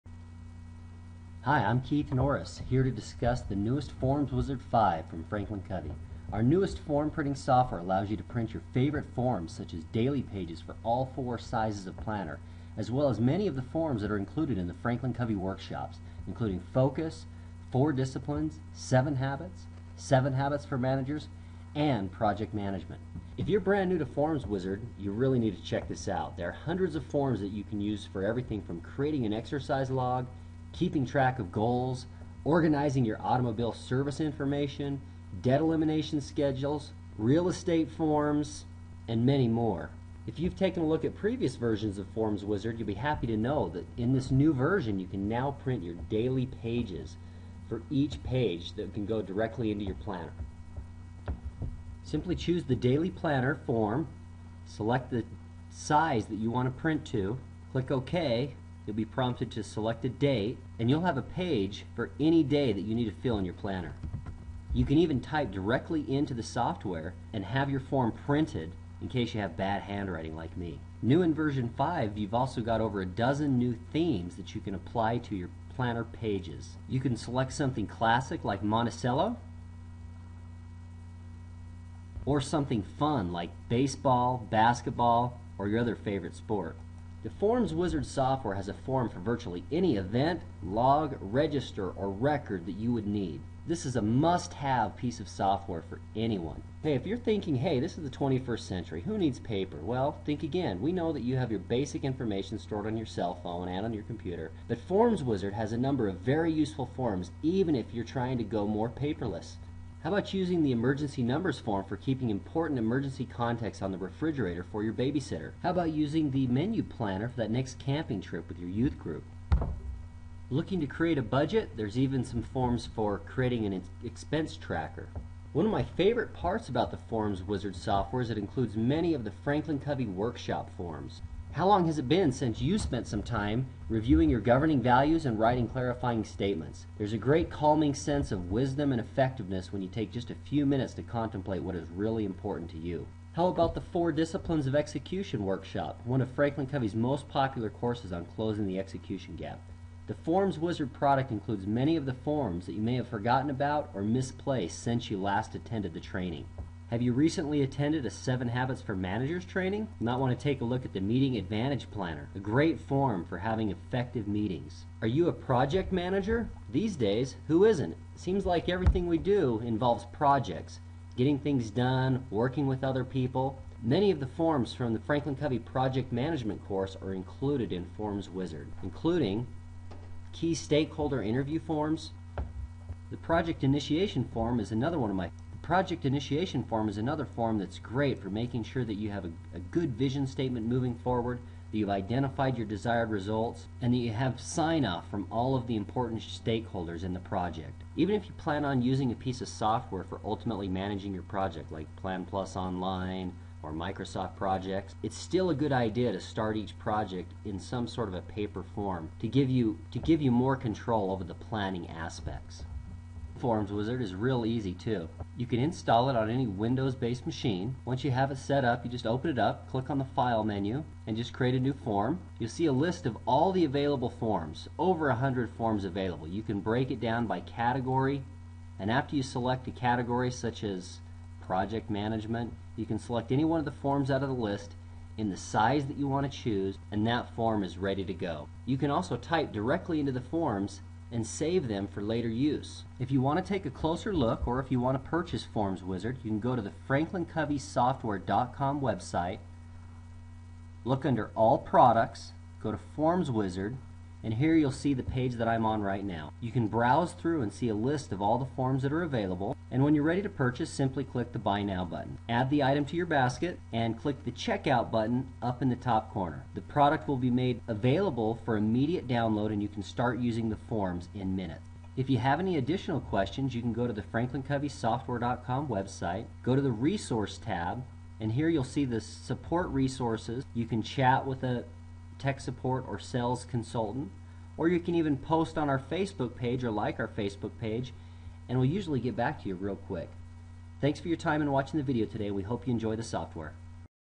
FormsWizard 5.0 Overview
Overview of FranklinCovey's newest form printing software. New in this version: Daily Pages, Monthy Calendars, Better than a yellow pad, New themes.